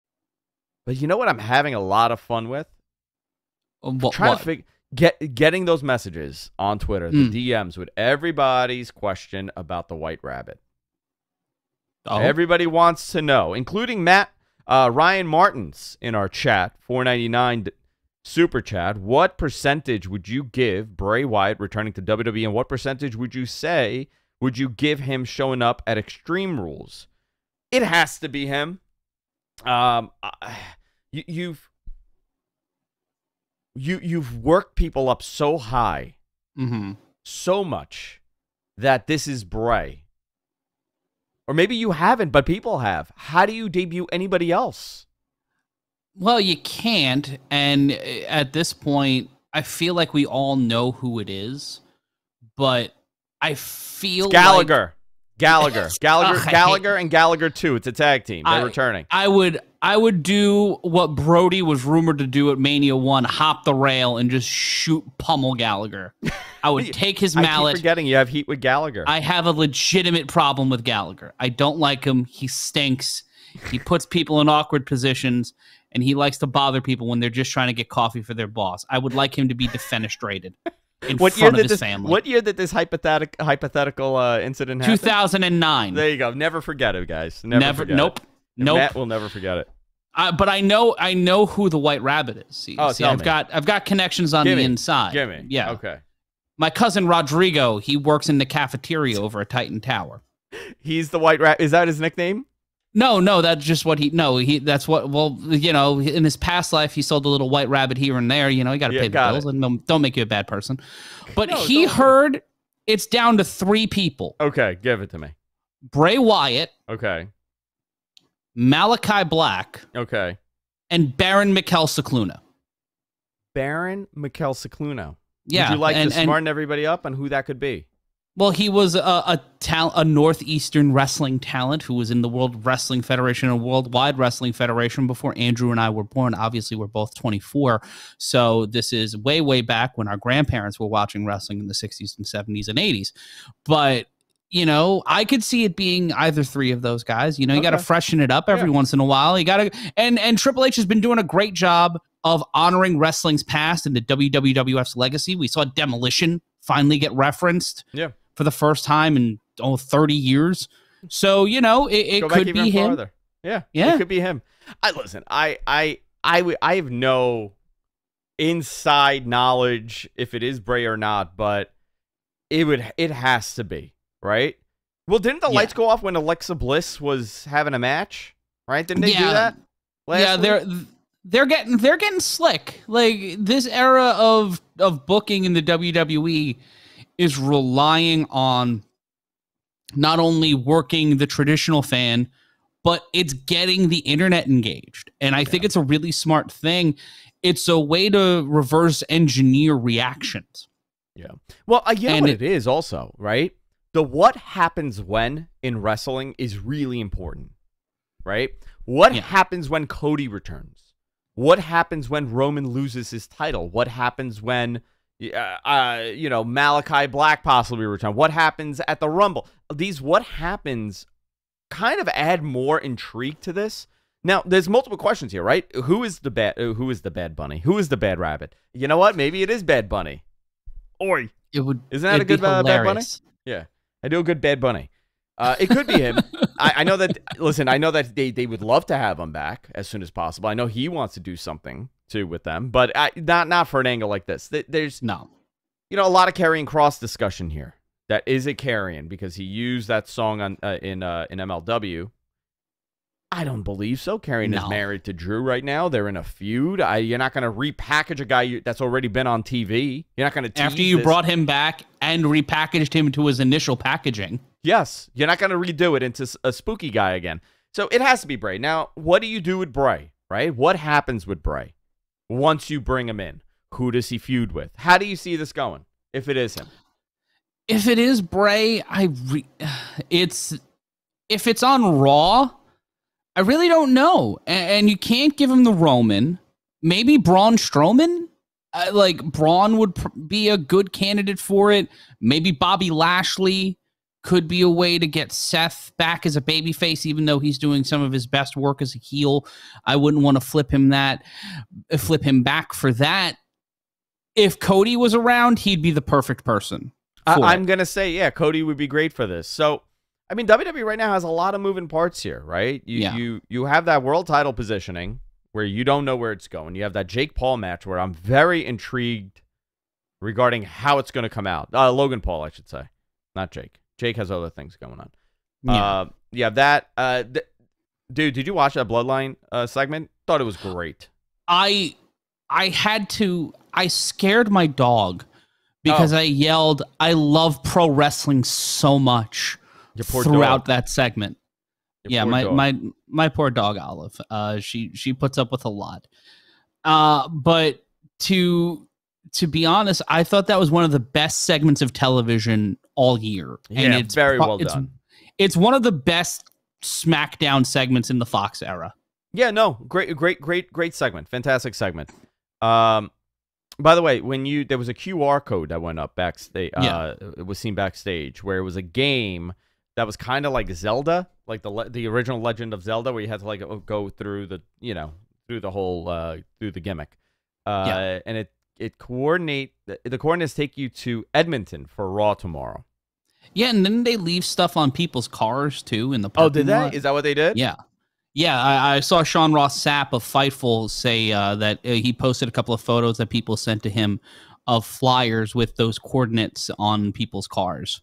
The White Rabbit: It’s Bray... Right...?
Andrew and Guest host Matt Ryan discuss the likelihood that WWE’s ‘White Rabbit' tease is anything other than Bray Wyatt!